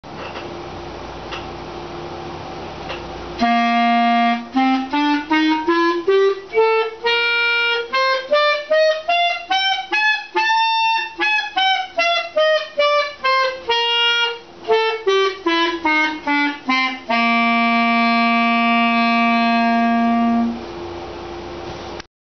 Clarinet Concert B Flat Major Scale
This is two octaves of my playing the concert C Flat major scale on my clarinet!